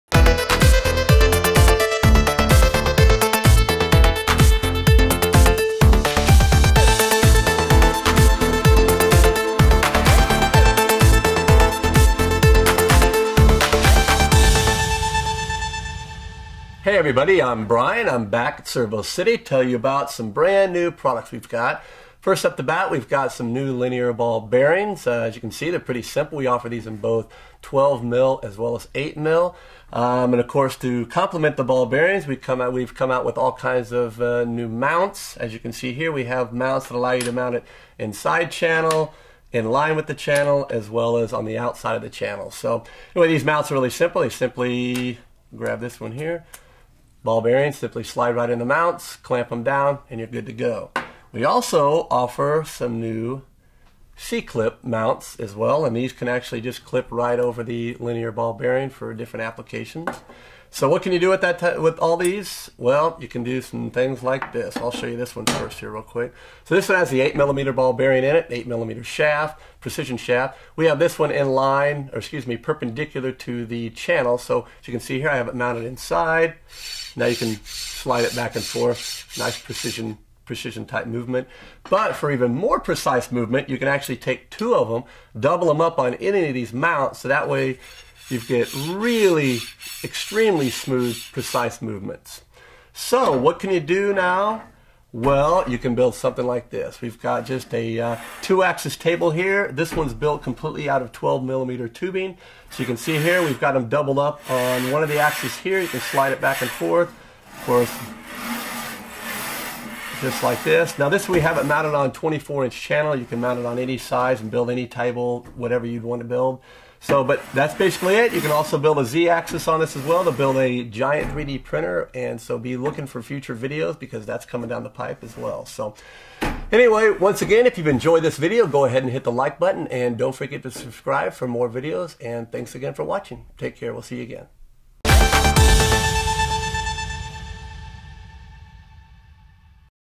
ServoCity Products: Linear Ball Bearings and Mounts
This week Brian talks about our new Linear Ball Bearings and the compatible mounts!